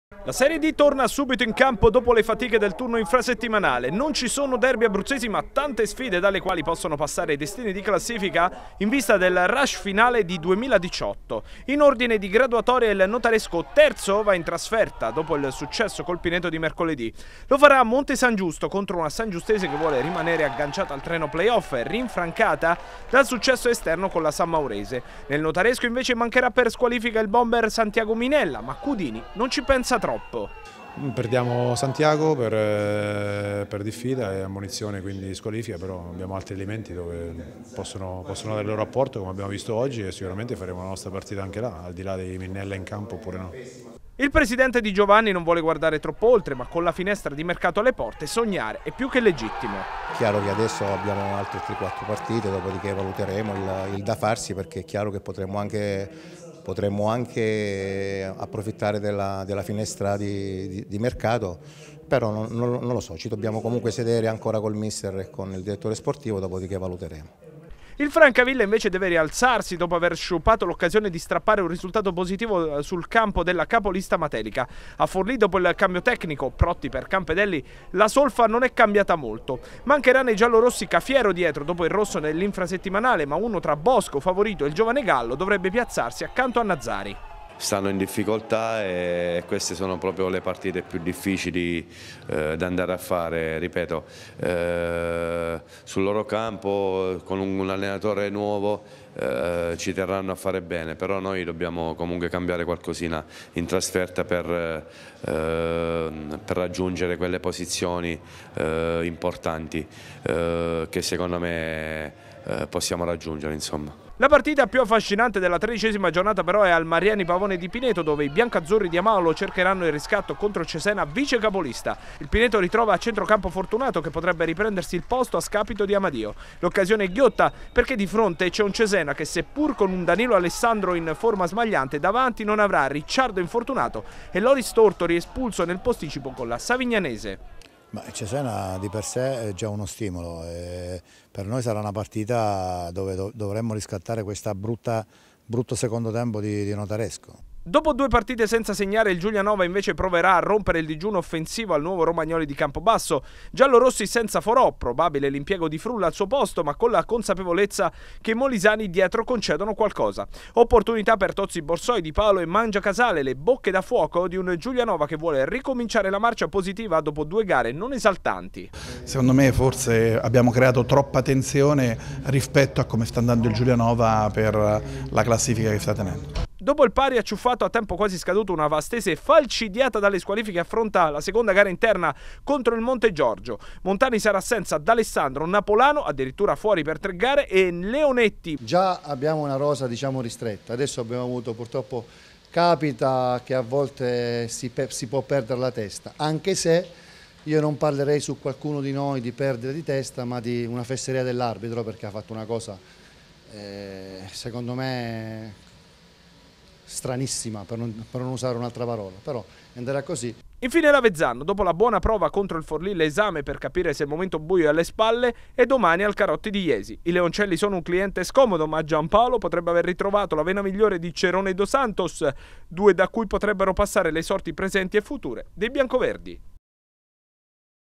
La Serie D scende di nuovo in campo
In Serie D nessun derby abruzzese ma tante sfide importanti per la classifica. Il clou al "Mariani - Pavone" che sarà tutto esaurito per la sfida tra il Pineto e il Cesena. La Vastese alla seconda consecutiva in casa mentre le altre giocano tutte in trasferta